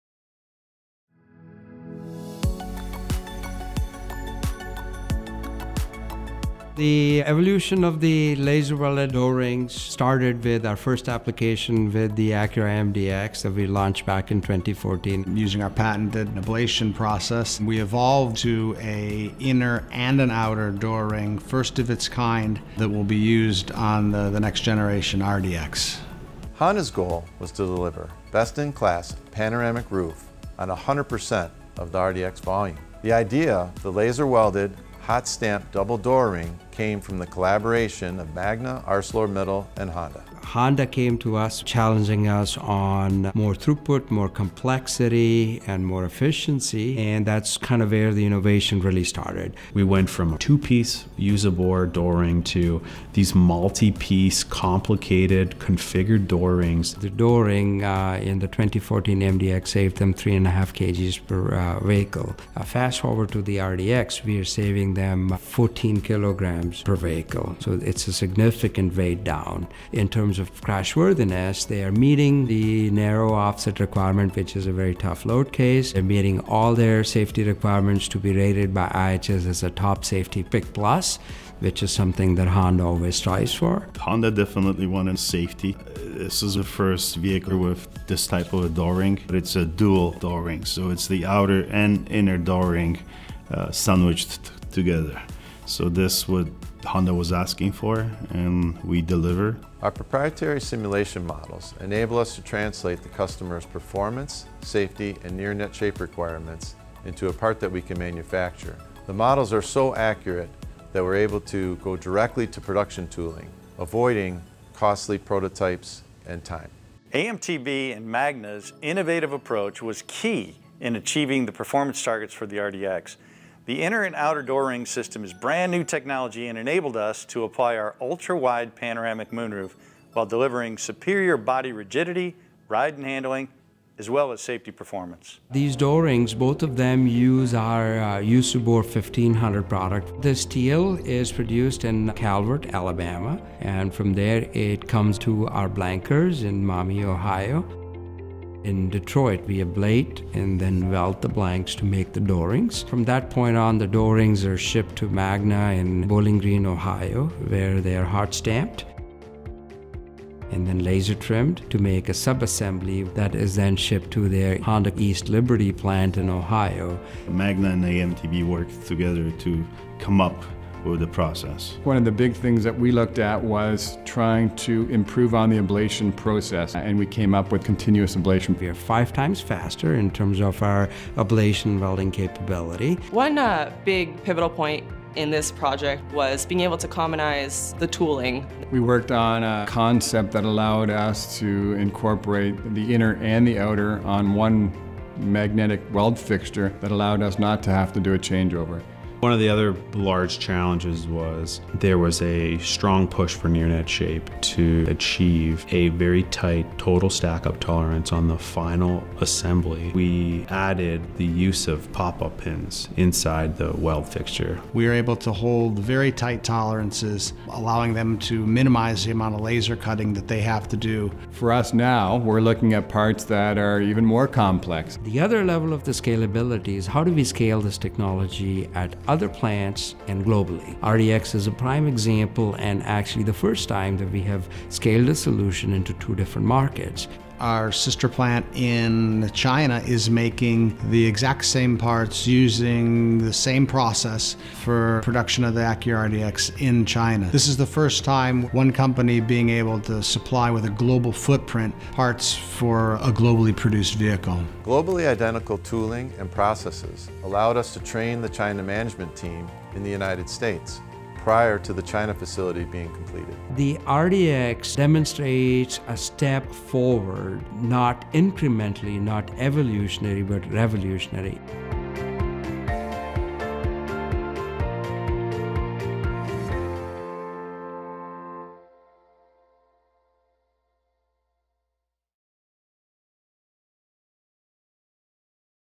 Inner and outer door ring system earns hardware
Utilizing nine total gauges of its patented press hardenable steel, ArcelorMittal, in collaboration with Honda R&D Americas and Magna International, created a hot-stamped, laser-welded inner and outer front door ring system for the 2019 Acura RDX. This lightweight door ring offers unprecedented safety, and its hardware earns hardware. It garnered a five-star rating for the narrow offset crash test and helped the RDX become an IIHS Top Safety Pick+. Additionally, the dual door ring was 2019 Automotive News PACE Award Finalist.